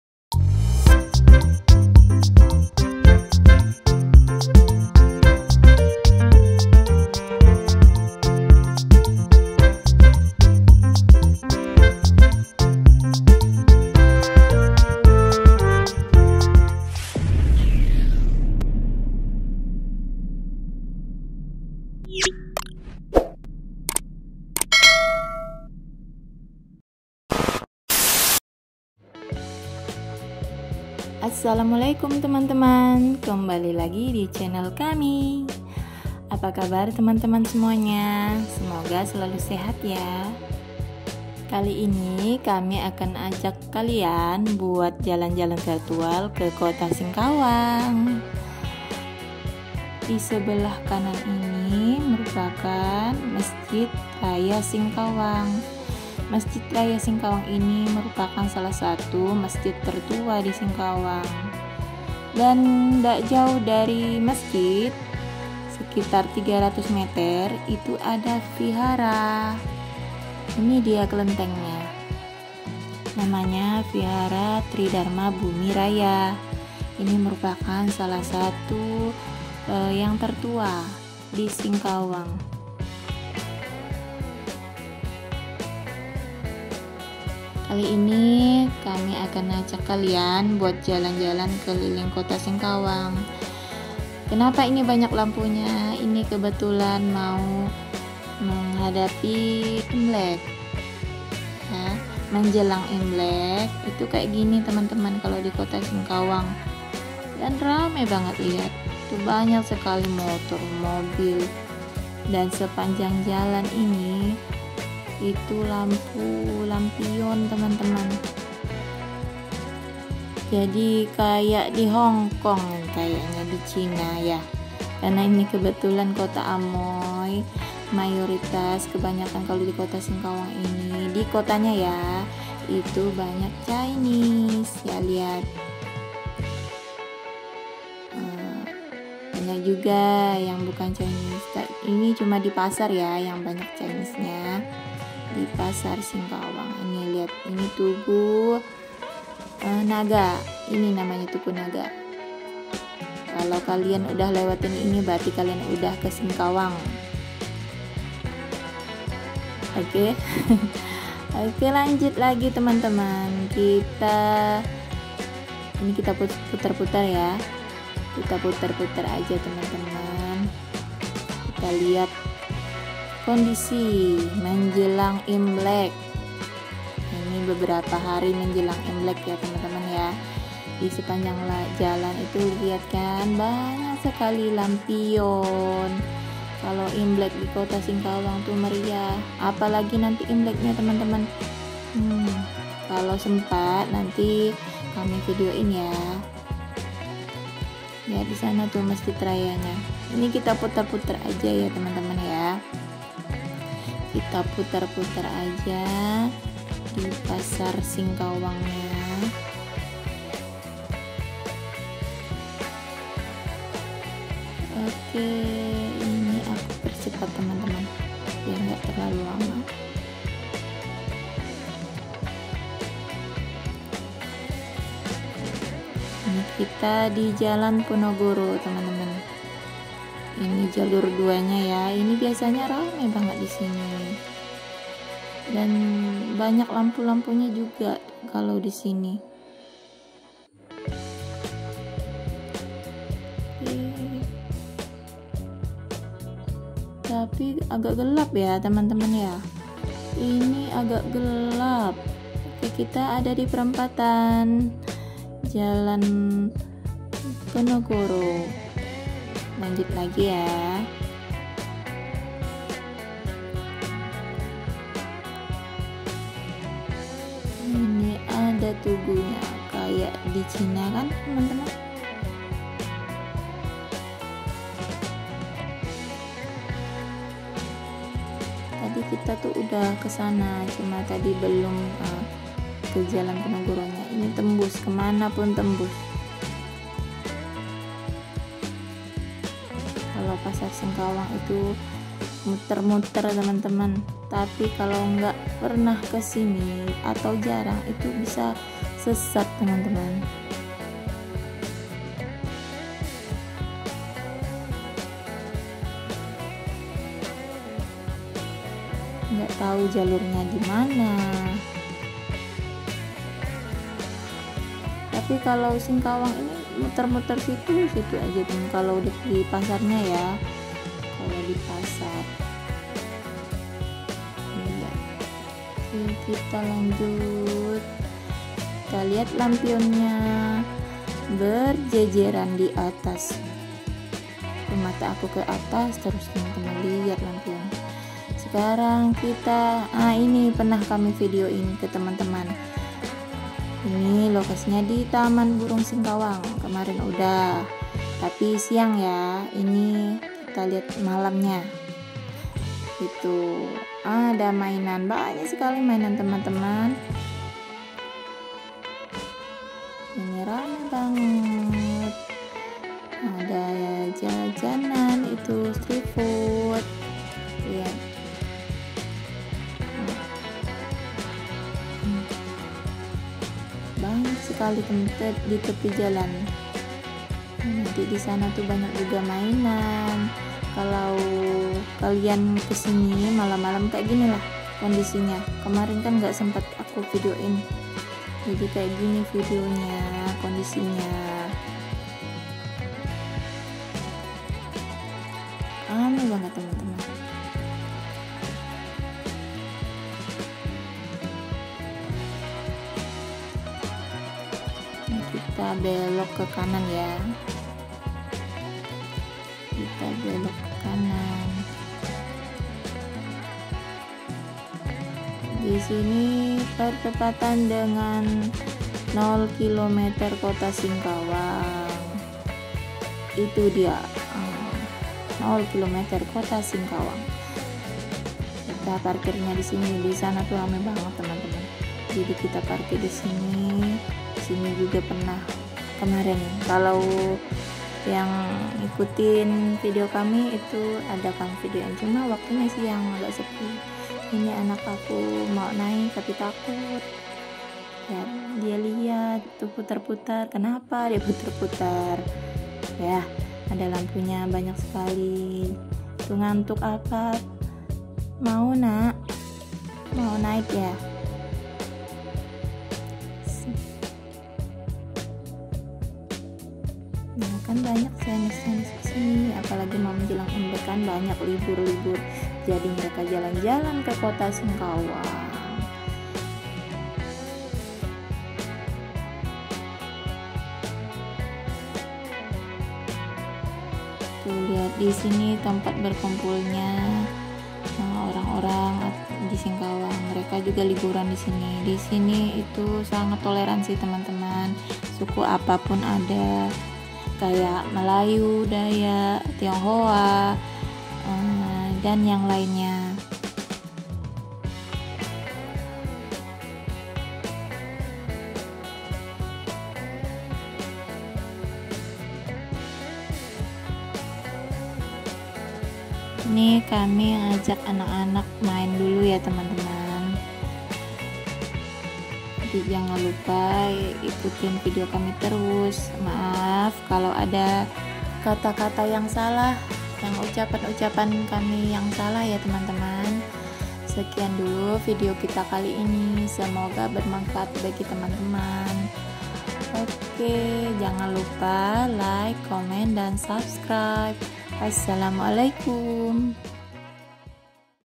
KONDISI MENJELANG IMLEK KOTA SINGKAWANG | BANYAK LAMPION JADI MERIAH
KONDISI MENJELANG IMLEK KOTA SINGKAWANG

video kali ini kami ngajak kalian jalan-jalan ke kota Singkawang. buat melihat kondisi menjelang Imlek. kemeriahan tidak hanya di rasakan masyarakat Tionghoa. Tapi yang lain juga ikut merasakan dan menikmati keindahan kota Singkawang. sangat toleransi bukan.

imlek 2022, imlek di china, imlek singkawang 2023, imlek 2023 tanggal berapa, imlek 2023, imlek